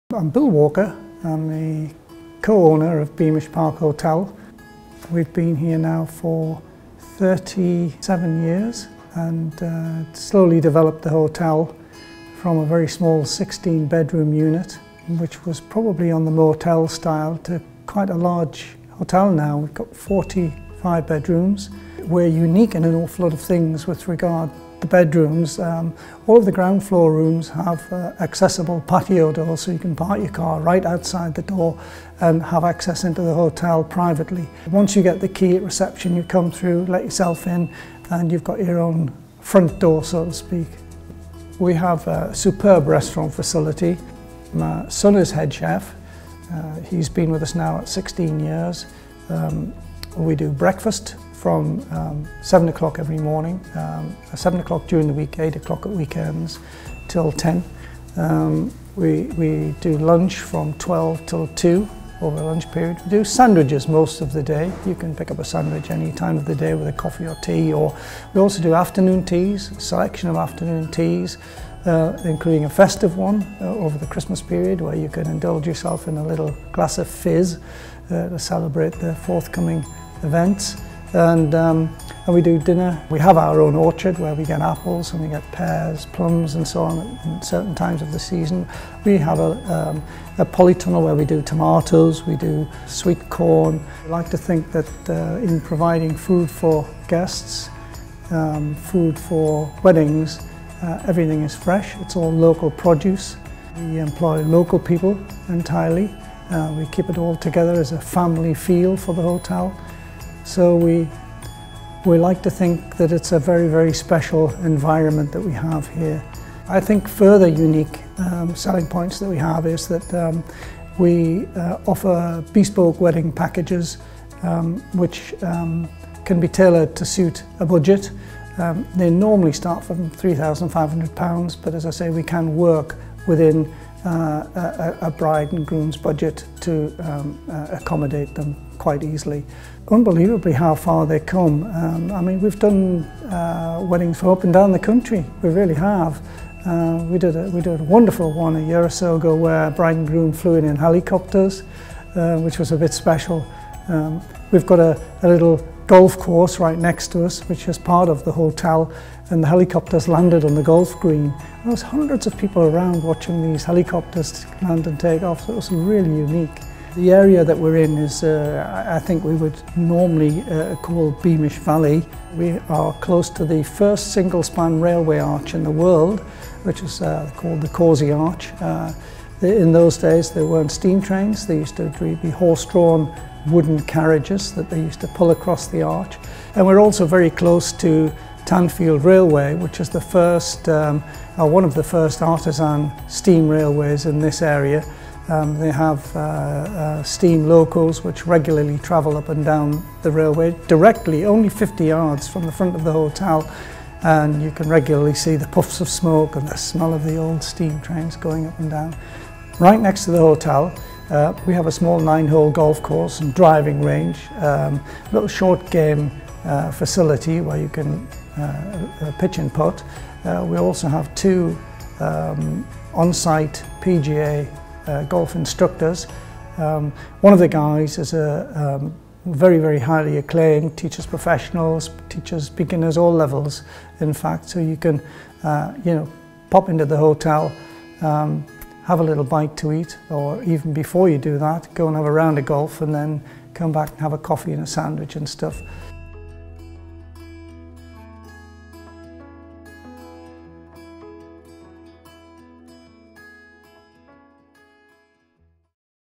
Beamish Park Hotel - Love Beamish and Stanley
In the heart of the Causey countryside is the fantastic Beamish Park Hotel. This family run hotel has so much to offer including amazing food in their restaurant, meeting spaces and of course weddings. It attracts guests from all over the UK to explore our fabulous countrtyside.

They pride themselves on their personal service ensuring that guests can relax and enjoy their surroundings. We spoke to owner Bill Walker to find out more.

Additional Photos: Love Photography